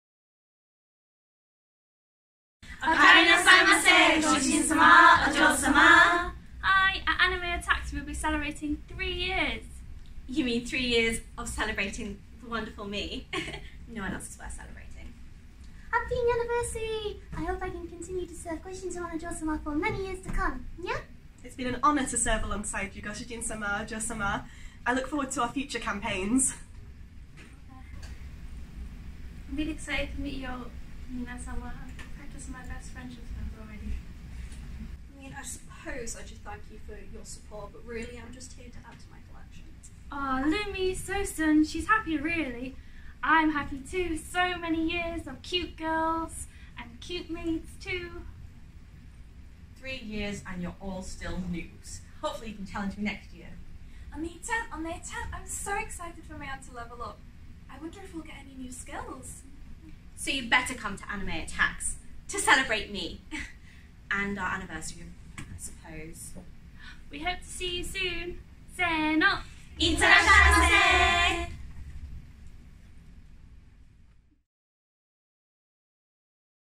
♥ Happy 3rd Birthday! ♥ Meian Maid Cafe @ Anime Attacks 9
Meian Maid Cafe will be celebrating their third anniversary at Anime Attacks 9 on 19th Oct 2019! The Meian Maids have a special message for all the Goshujin-sama and Ojou-sama who have supported them thus far, and all those who will in the future!

We hope you can return home to us at Anime Attacks, Gateshead, to celebrate! いってらっしゃいませ!